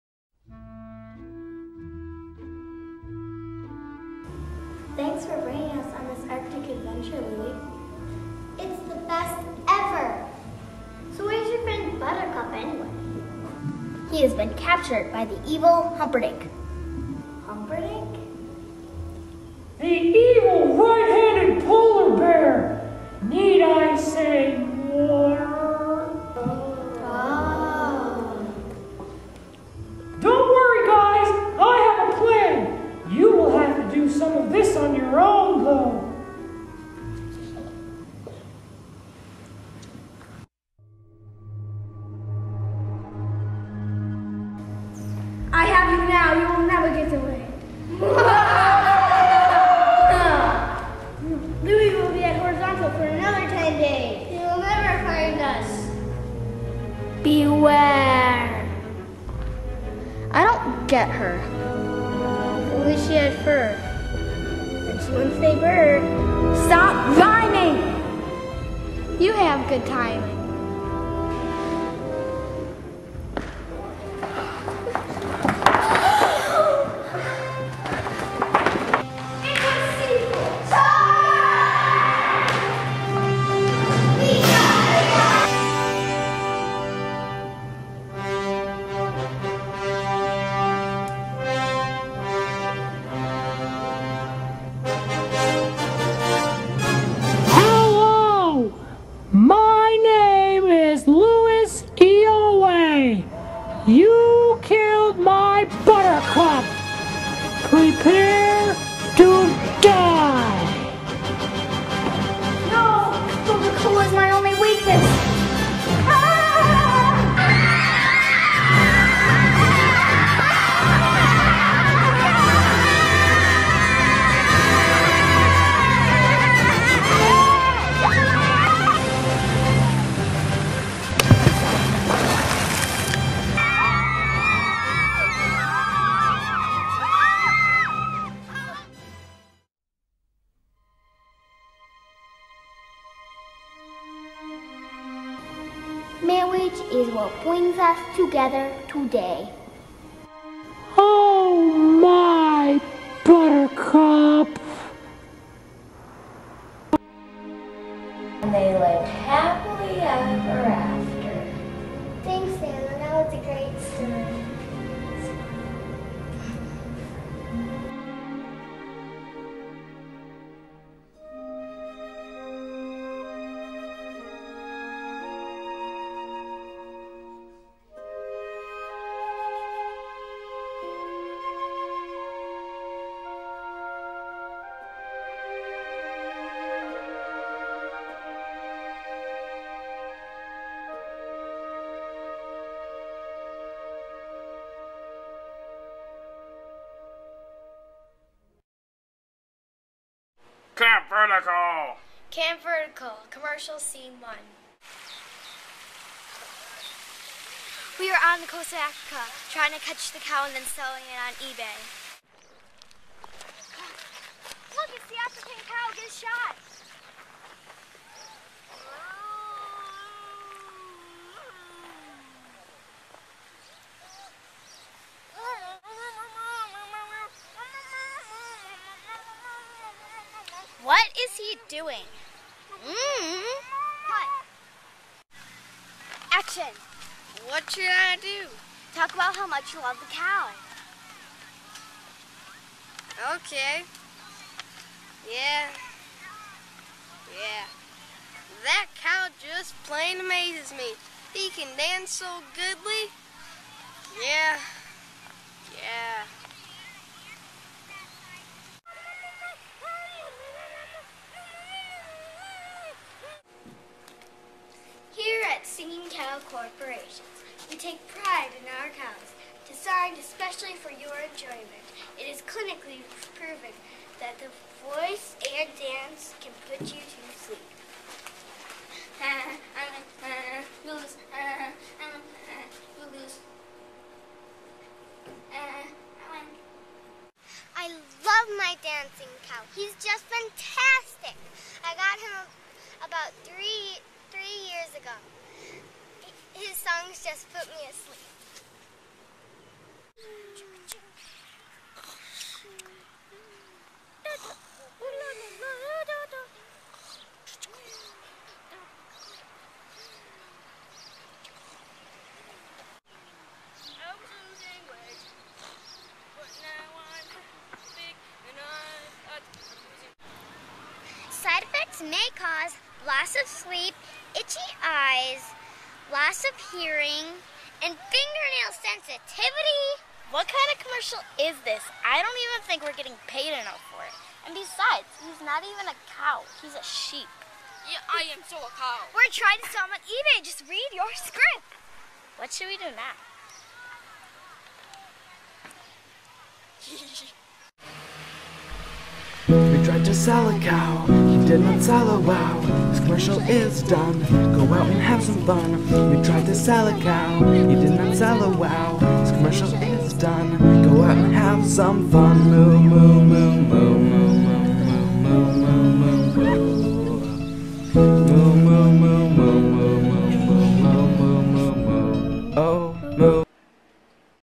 2009 Camp Vertical LCA--2
Part 2 of the movie made by Camp Vertical 2009 Lights Camera action crew at Elmbrook Church